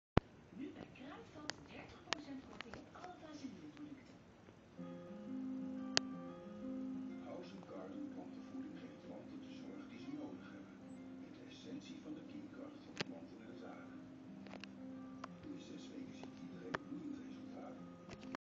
bad doggy
She's trying to get on him, but got the wrong side of him haha! Yes, SHE ghehe